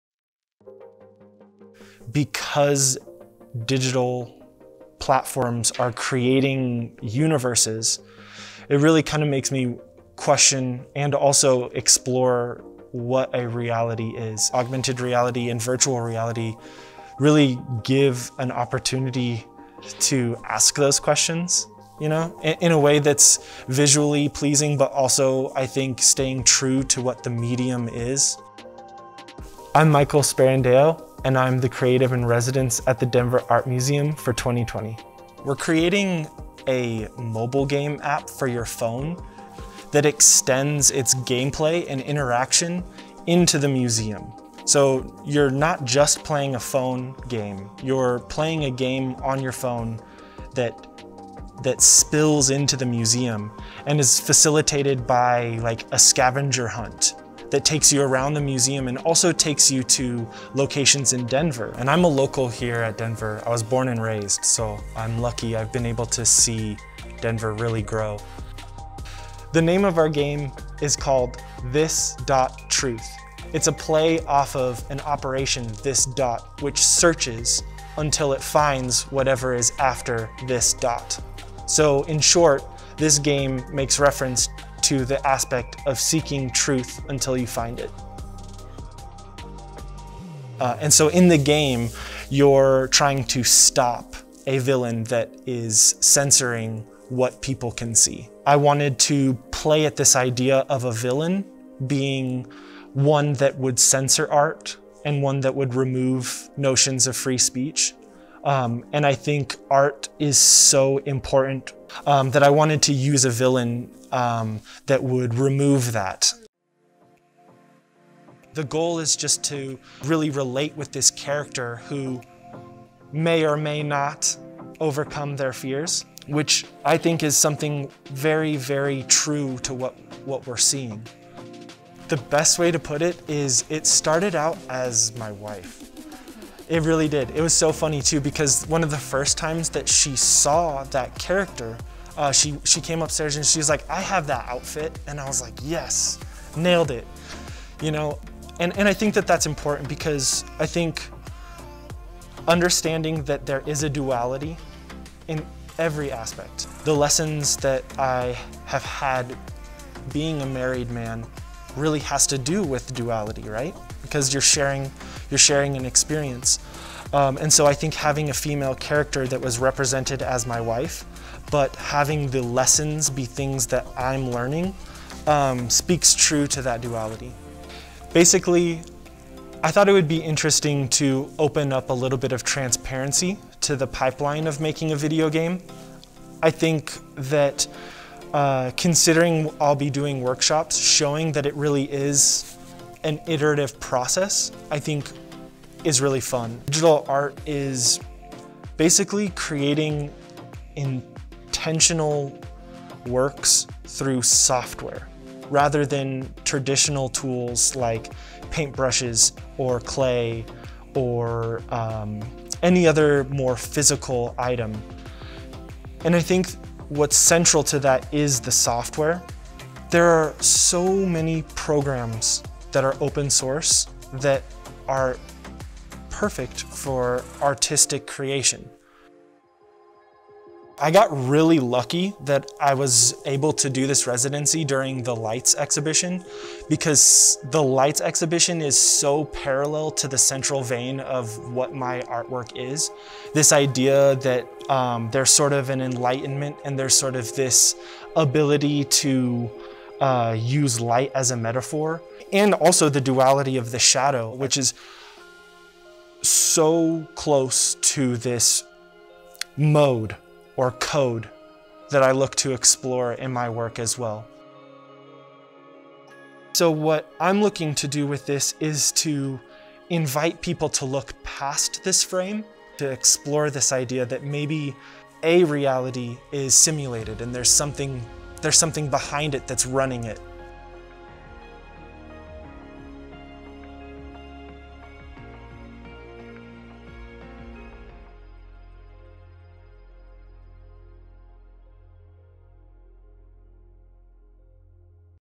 Digital Artist Michael Sperandeo Discusses Mobile Video Game about Art & Truth
The Denver Art Museum's 2020 Creative-in-Residence is Michael Sperandeo, a local creative who uses new media as a platform.

During his residency at the DAM, Michael created a digital game, "this.truth," which encourages players to interact with the museum in both a physical and virtual space. In short, the game makes reference to seeking truth until you find it. In the game, you're trying to stop a villain who's trying to censor what people can see.